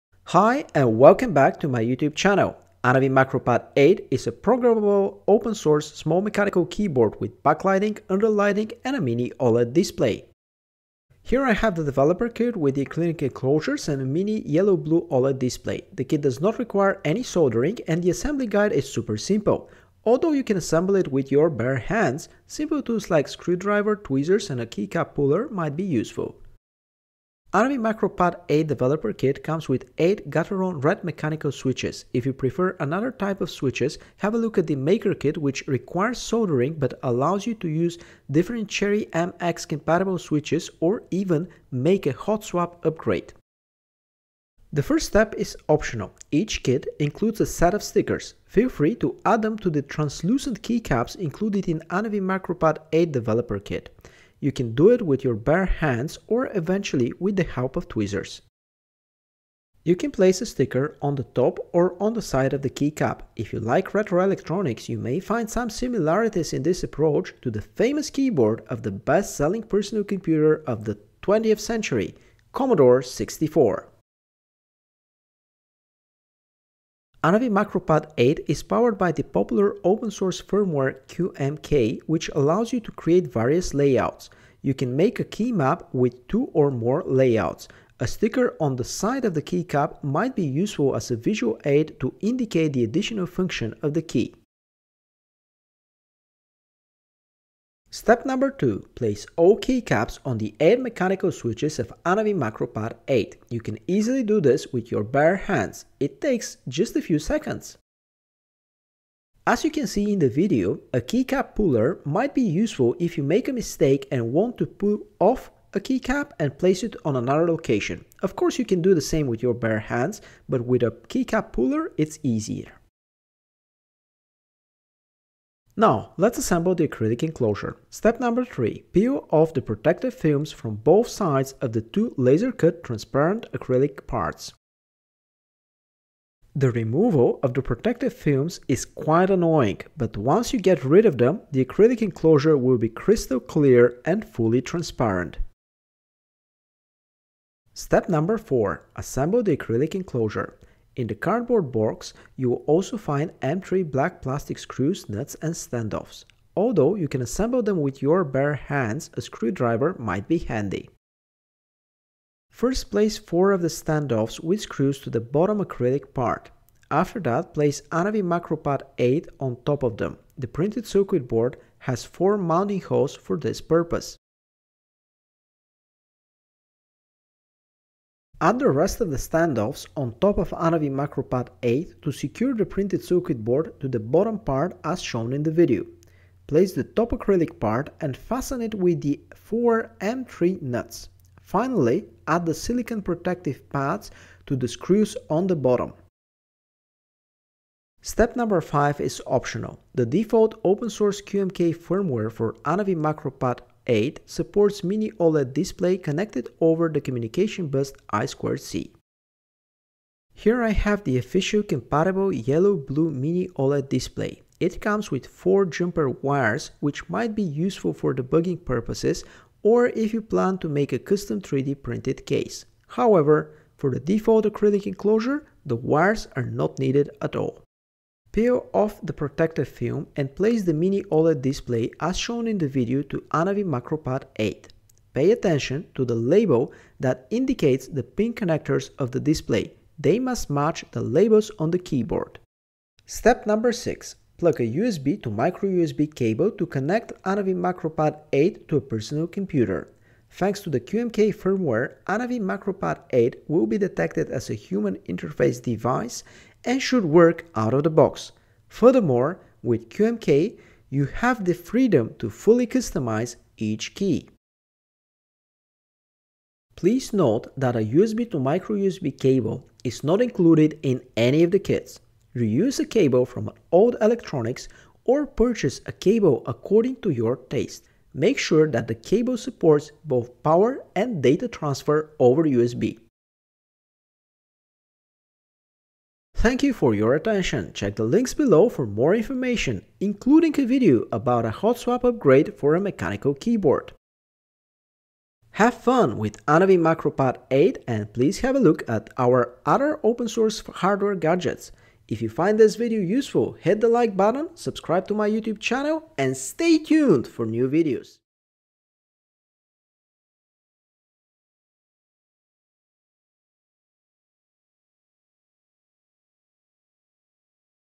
ANAVI Macro Pad 8 Developer Kit Assembly Guide
Step by step instructions how to assemble ANAVI Macro Pad 8 Developer Kit.

0:00 Unboxing
0:44 Step 1: Stickers
1:37 Step 2: Keycaps
2:04 Step 3: Protective Films
2:27 Step 4: Acrylic Enclosure
3:18 Step 5: Display
4:02 Step 6: Turn On